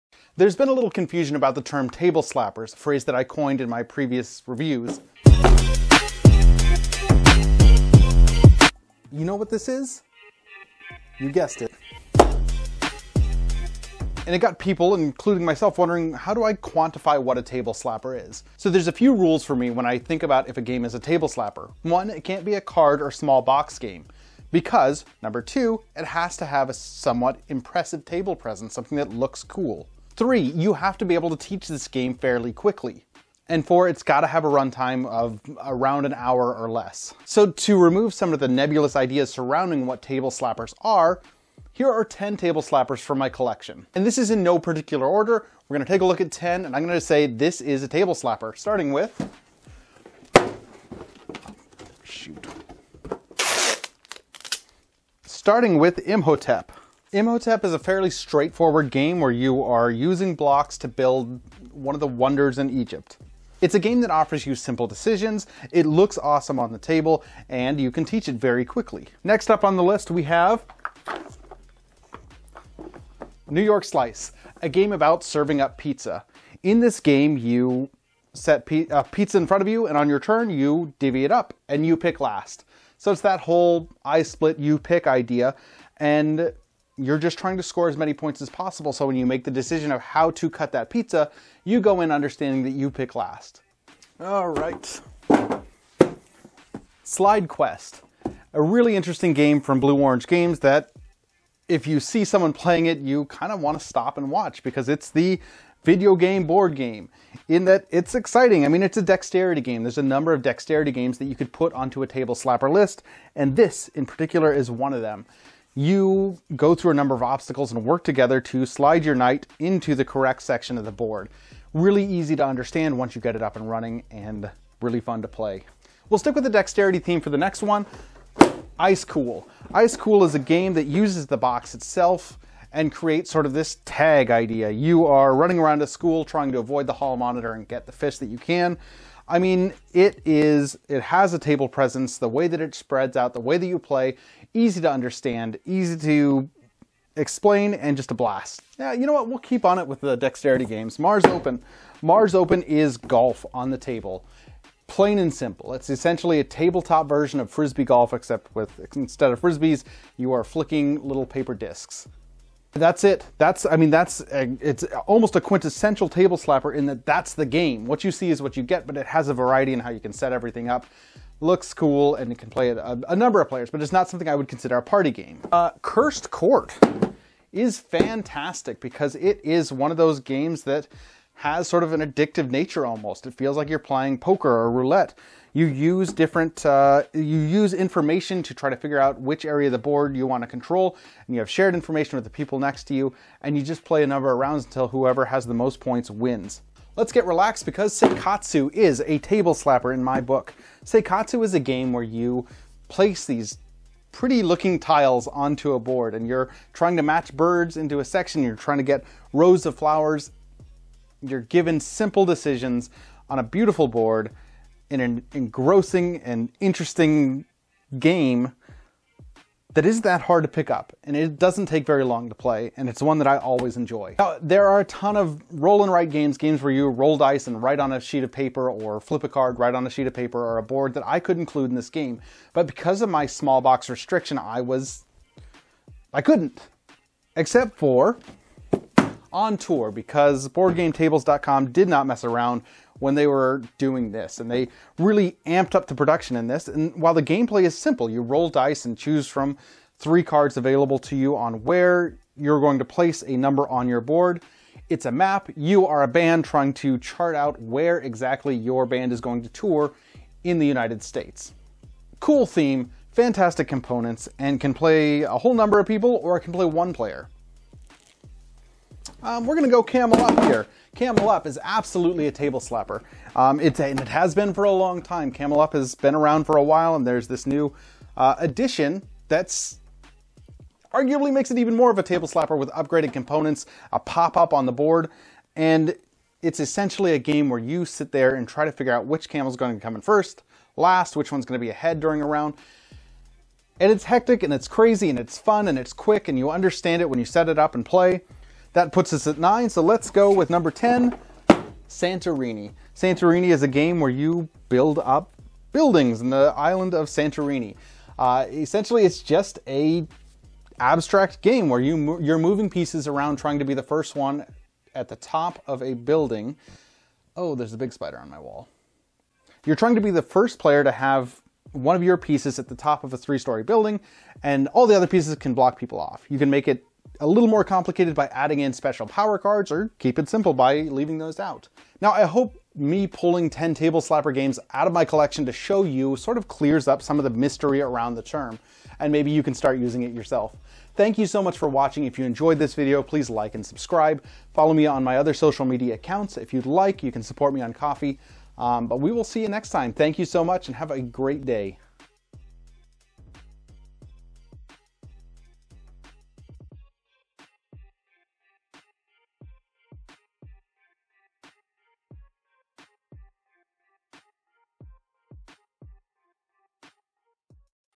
10 BOARD GAMES FOR ANY OCCASION -- Table Slappers
We're diving into the term "Table Slappers" in regards to board games, so buckle up as I look at 10 direct from my own shelves.